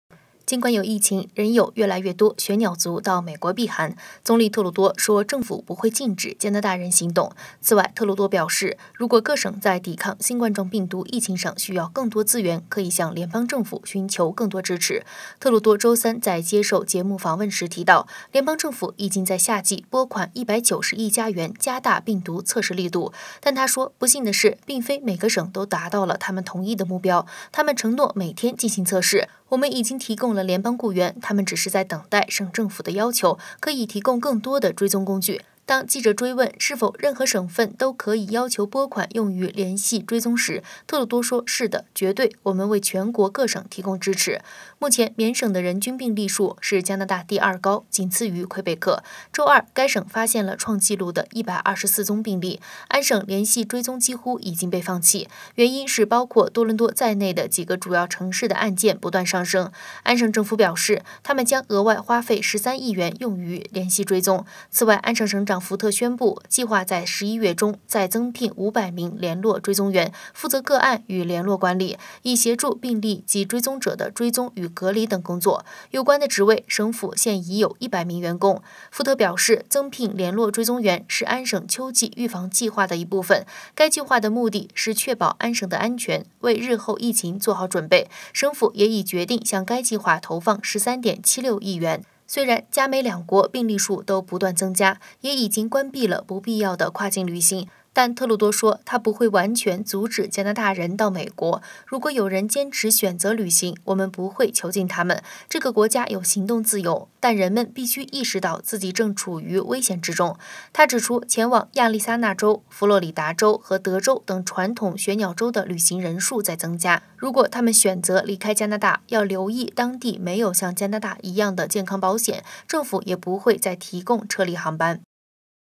越来越多加拿大人去美国避寒 特鲁多警告但不禁止
尽管有疫情，仍有越来越多雪鸟族到美国避寒。总理特鲁多说政府不会禁止加拿大人行动。此外，特鲁多表示，如果各省在抵抗新冠状病毒疫情上需要更多资源，可以向联邦政府寻求更多支持。